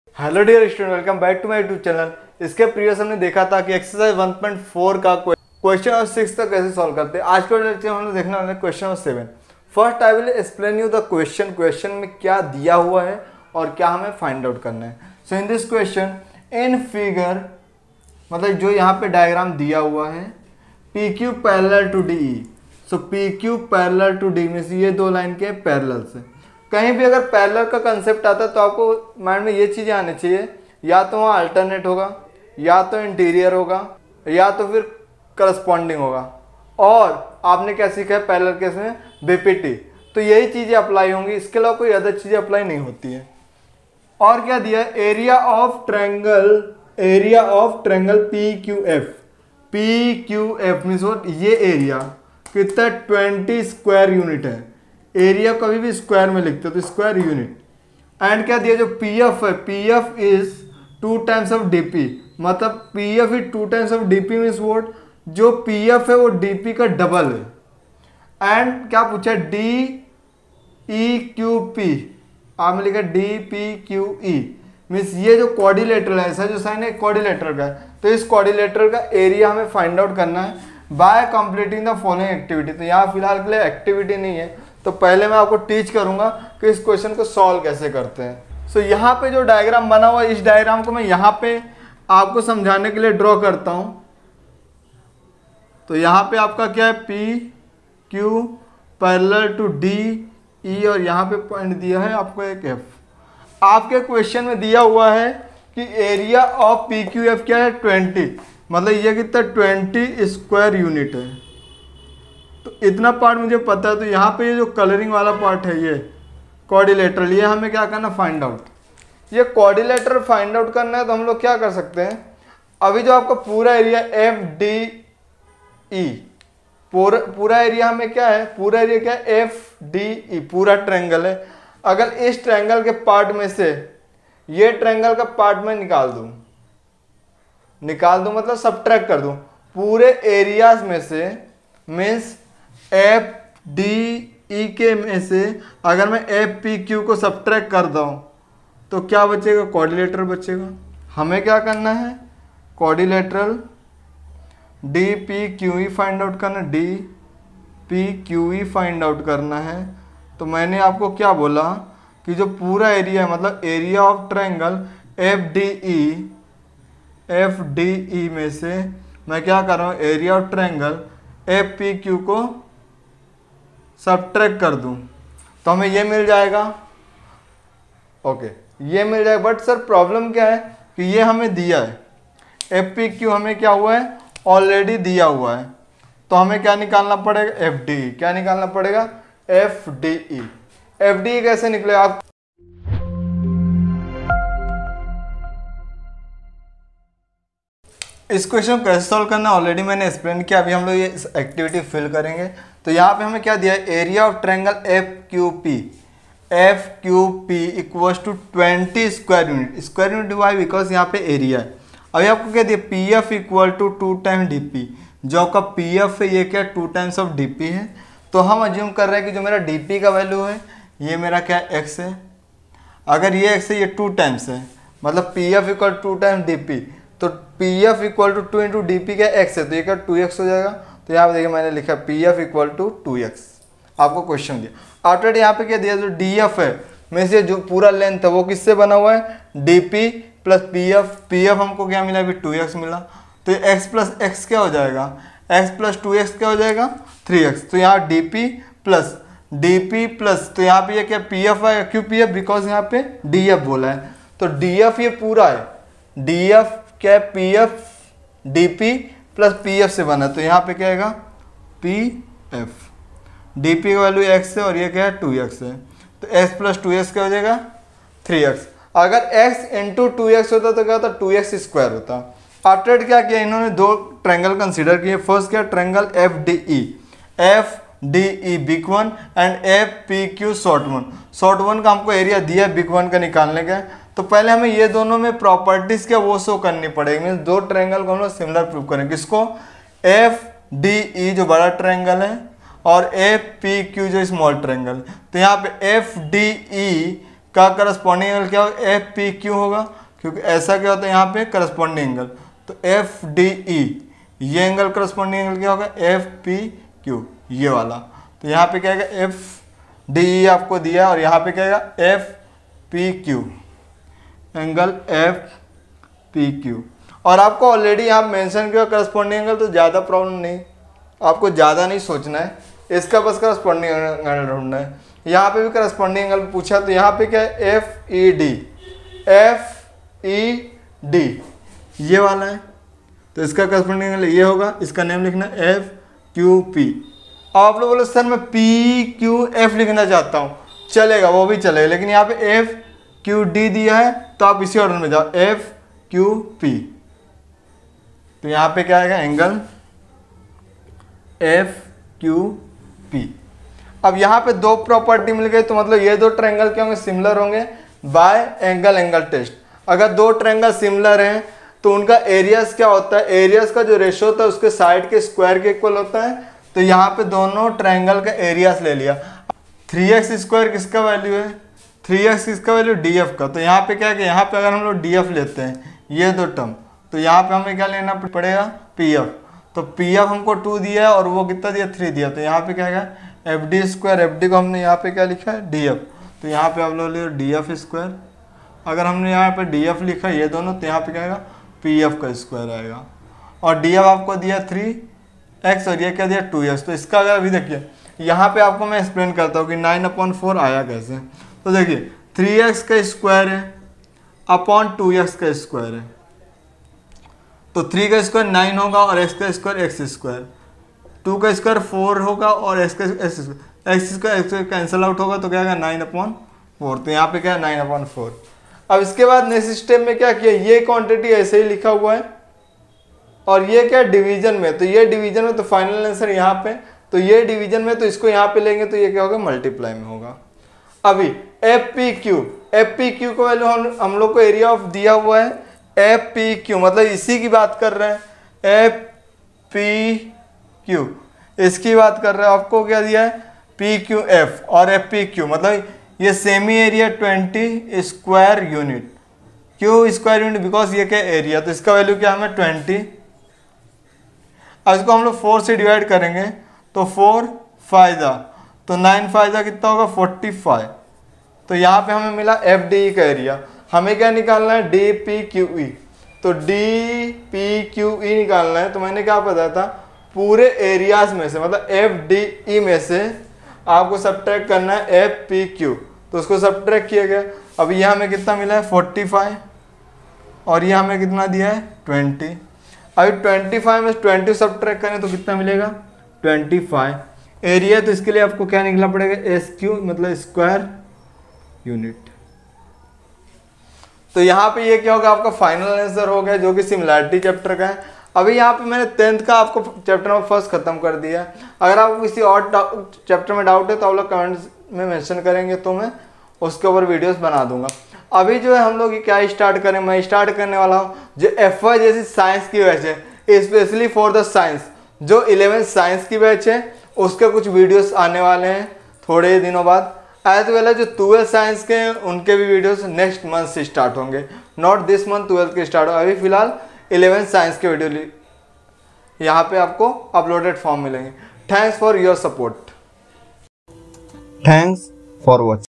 Geometry || Chapter 1 Similarity || Exercise 1.4(Part 4)|| By Vimlesh Sir
In this video we will learn about question based on similarity of triangle ...

Learn & Grow

Regards and love
Vimlesh sir